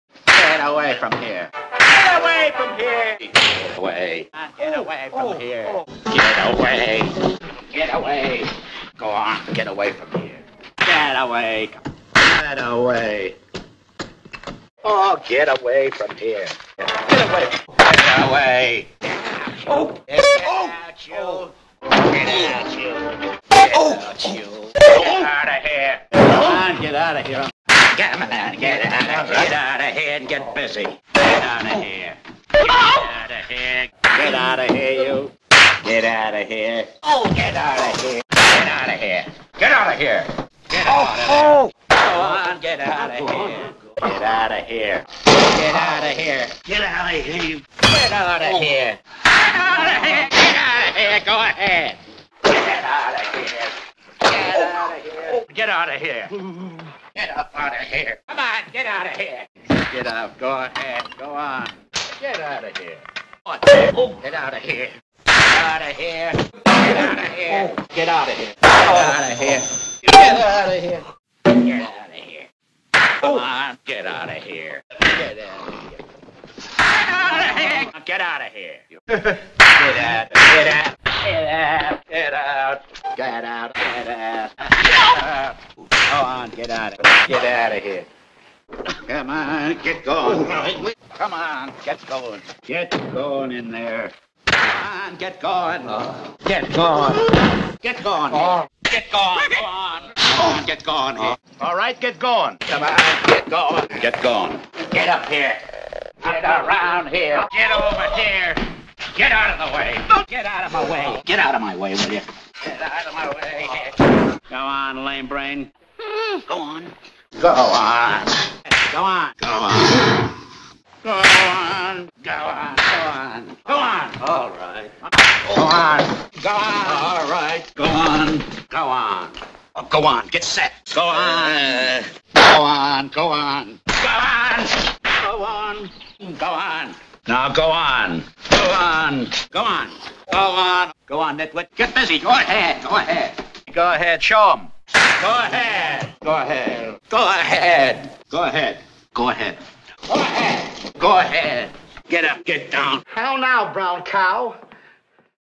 Get Away, Get Outa Here, Get Going, Go On & Go Ahead!
Variants of same basic catchphrase. Usually accompanied by a slap, often follows delivery of a punchline. Mostly Moe.